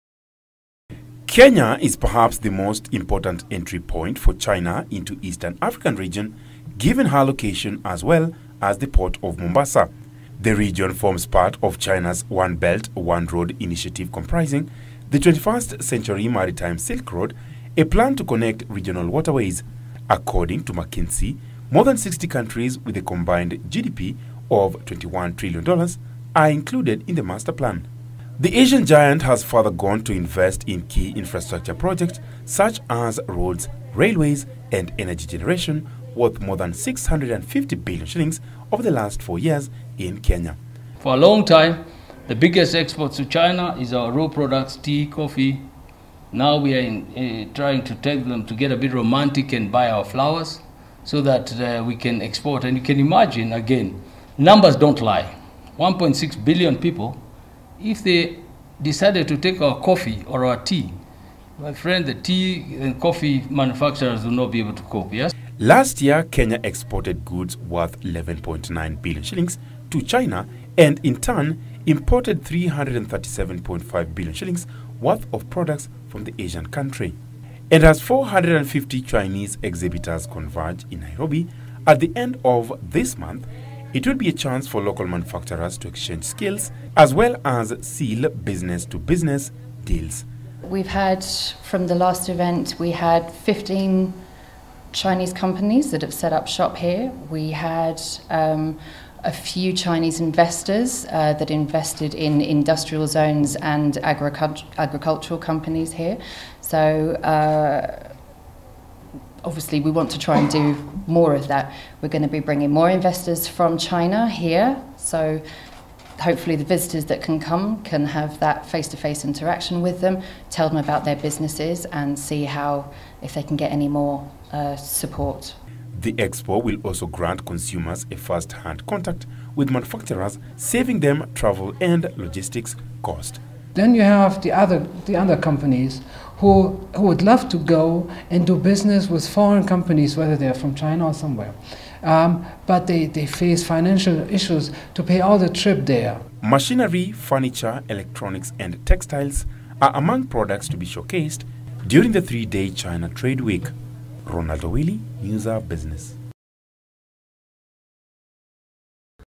Local manufacturers urged to engage Chinese counterparts to gain skills
Local manufacturers are being challenged to engage their Chinese counterparts to gain skills and knowledge that will enable them make quality products. This could be through expos attended by Chinese exhibitors.